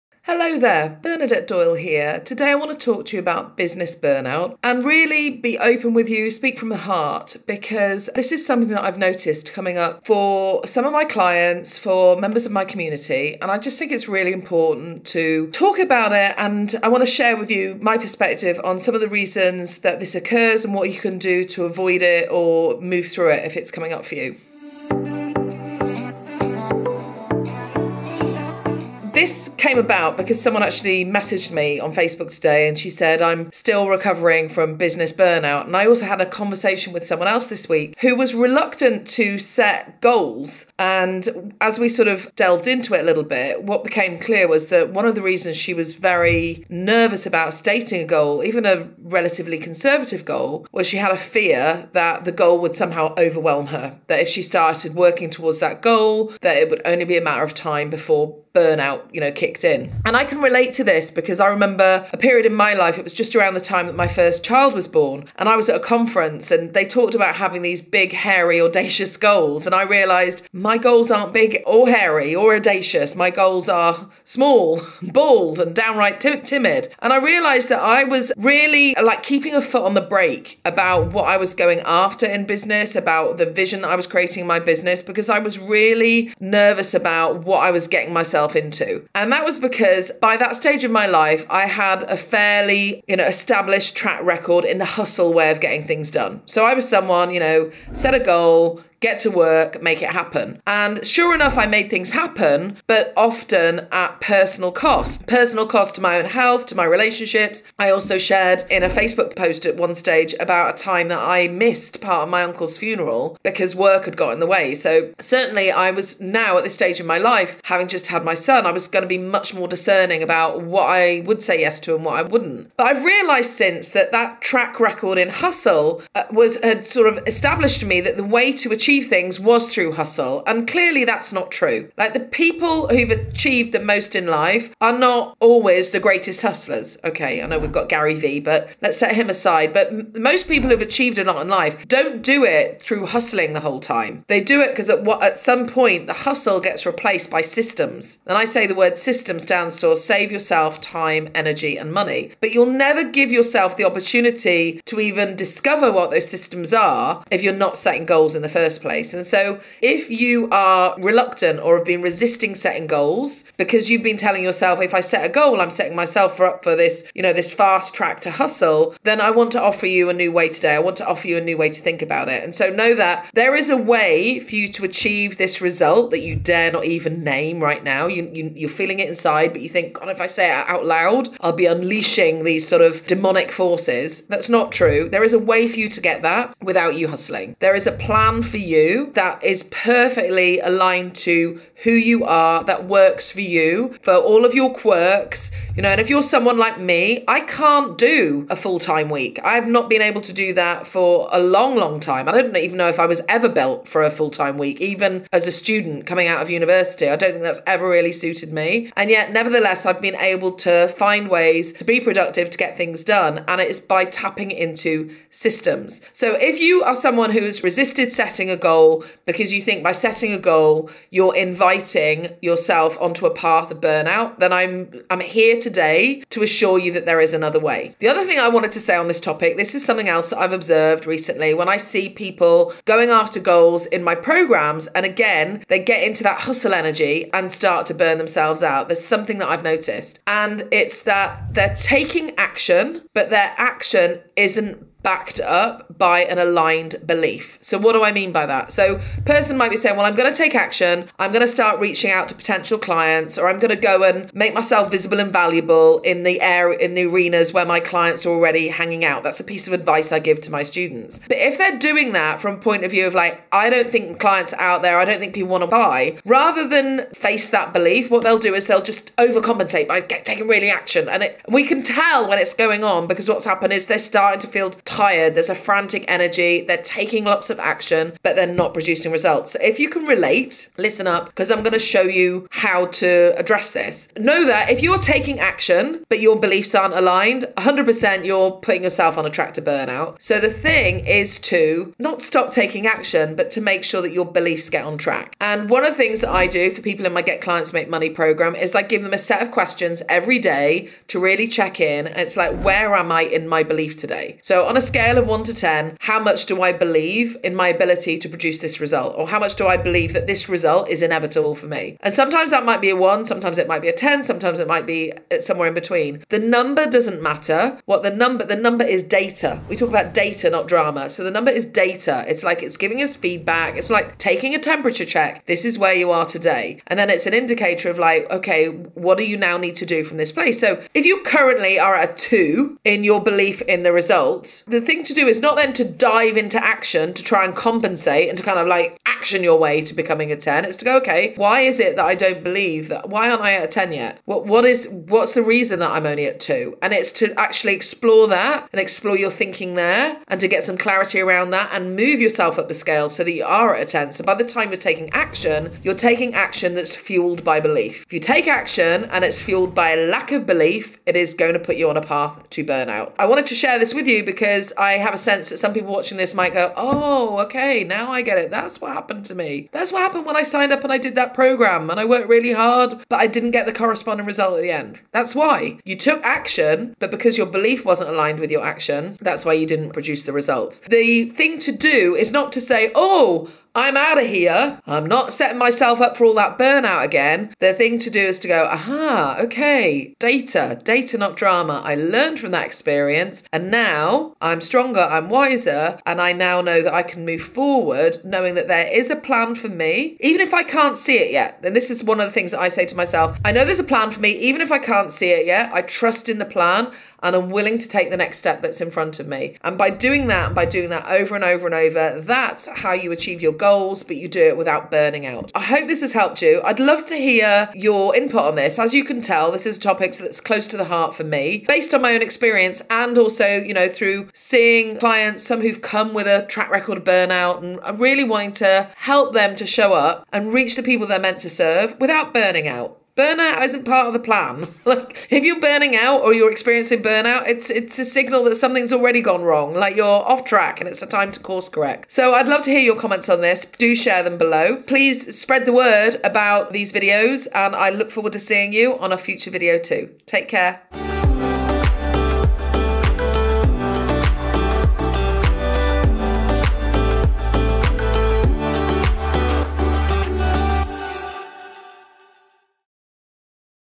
Are you Afraid of Business Burnout
Are you afraid of business burnout? In this video I share how to avoid it and what to do if you're already feeling it. You're not alone if you are feeling overwhelmed. Bernadette Doyle has some great tips on how to avoid burnout in her video "Are You Afraid of Business Burnout?" Watch this video and learn the key things you need to know about business burnout.
Are you afraid of business burnout? If so, then this video is for you. I'm here to offer some tips on how to avoid the "burn out" phenomenon that plagues many entrepreneurs. Watch this video and learn about four simple things you can do right now to prevent business burnout!

Want a plan to get more coaching clients and make more money? Download my FREE guide 'The ABC of Consistent 10k+ Months'